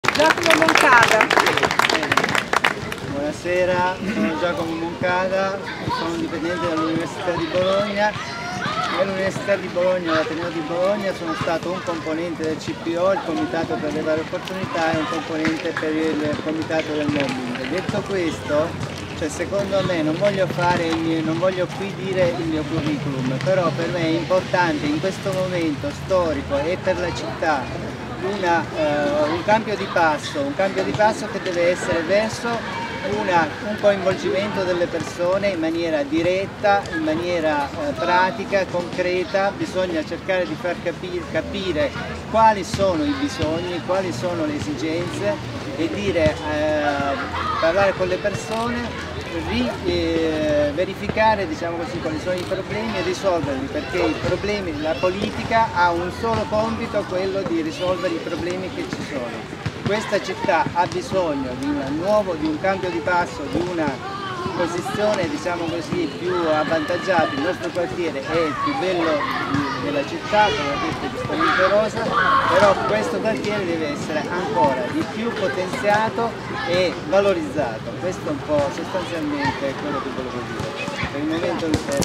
Presentazione candidato del centrosinistra Giacomo Moncada
Centrosinistra per Santo Stefano
presentazione delle nostre candidate e dei nostri candidati per il consiglio di Quartiere :

Candidata Presidente:
Amorevole Rosa Maria

Candidate/i consigligliere/i:

Almiropulo Anna Rosa
Bacchi Reggiani Giuseppe
Berti Nara
Bertini Silvia
Boni Lorenzo
Carbone Massimiliamo
Cavalieri Paolo
Costantini Cristiana
Genovesi Lorenzo
Malvi Cristina
Mazzù Anna Maria
Moncada Giacomo
Politi Andrea
Rampioni Patrizia